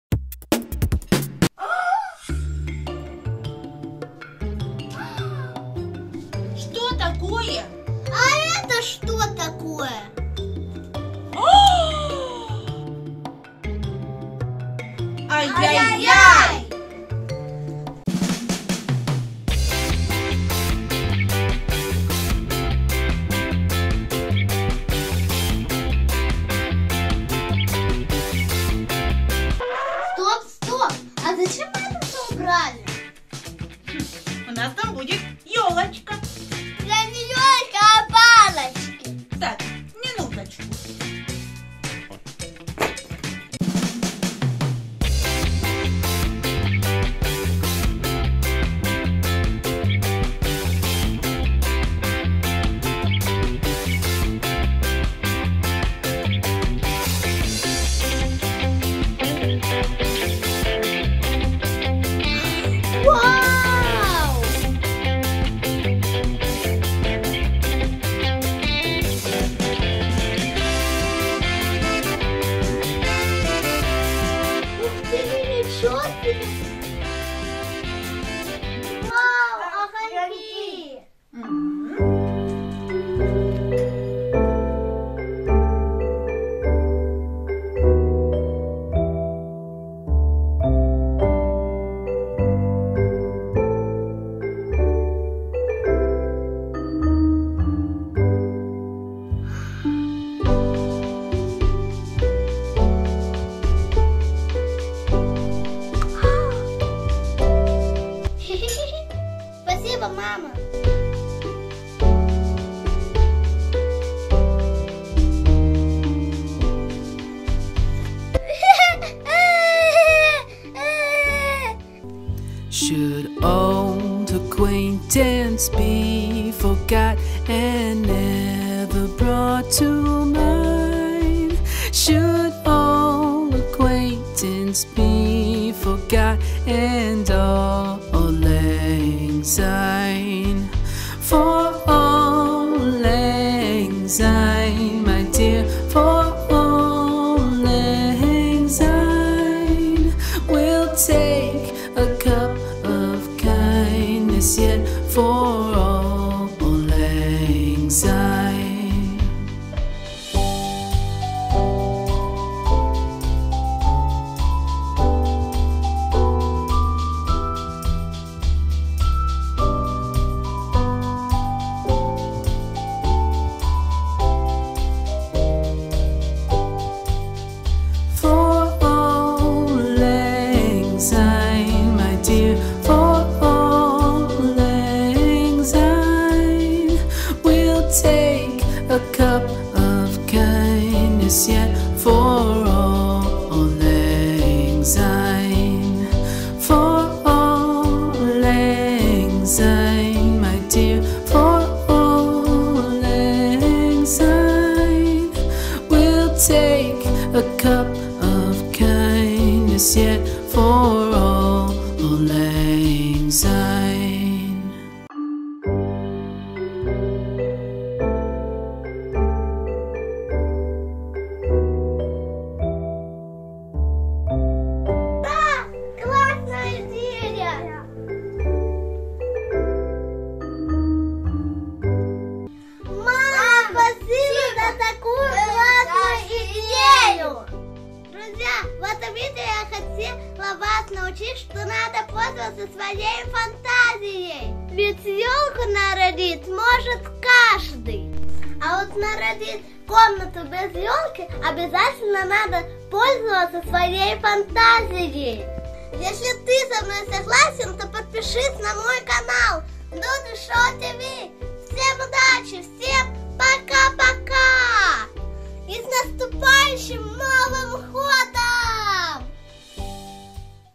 Учимся делать ЕЛКУ. Как украсить комнату к новому году 2022 своими руками. ТИГР - символ года 2022
Как сделать елку своими руками. Учимся делать и украшать новогоднюю елку и включать фантазию.